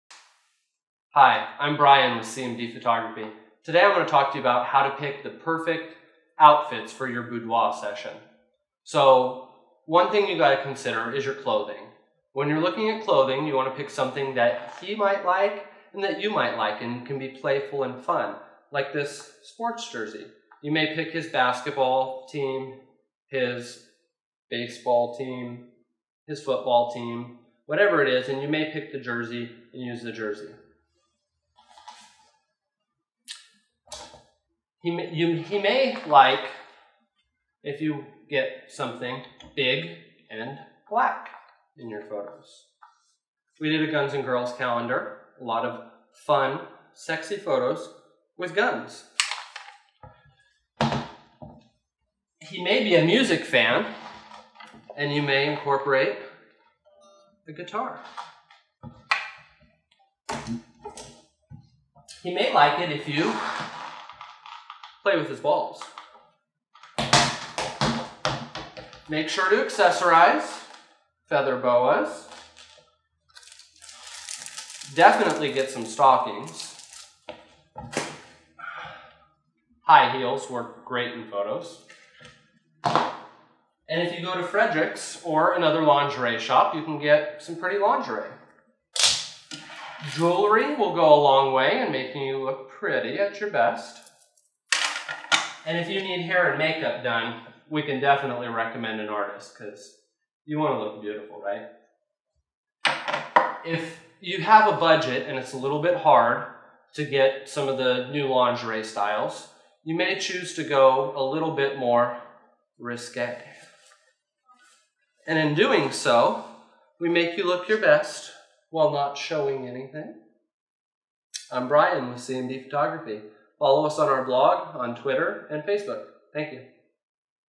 Boudoir Clothing Tips
What are some of the clothing choices you should make for a boudoir photography session? Bryan of C&B Pictures discusses a few of your options so you get a great boudoir photo.